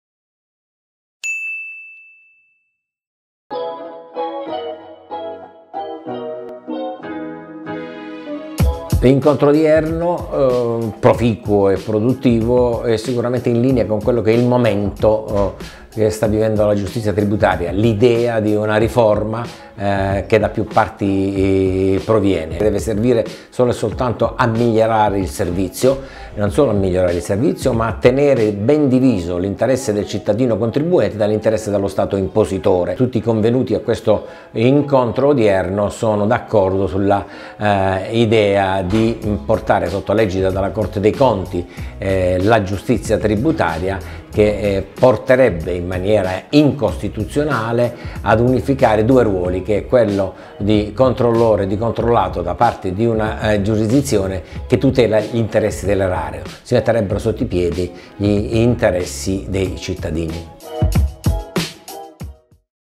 "Skill Breakfast. I caffè della competenza" su processo tributario - Antonio Leone
La video intervista dell'on. Antonio Leone, presidente del consiglio della giustizia tributaria, a margine del primo appuntamento degli “Skill breakfast. I caffè della competenza”. "Riformare il processo tributario: come?” è il titolo scelto per l'incontro d'esordio che si è tenuto lo scorso 7 novembre nella sede capitolina di The Skill, lo studio di comunicazione promotore dell'evento. Insieme al presidente Leone Sono intervenuti Francesco Giuliani (partner dello studio Fantozzi & Associati) e un panel selezionati di rappresentanti dell’Ordine dei commercialisti come Giuseppe Bilancia, Andrea Di Mise, Alberto Turini, Paolo Brescia, e avvocati tributaristi come Valentina Guzzanti e Giovanni Mameli.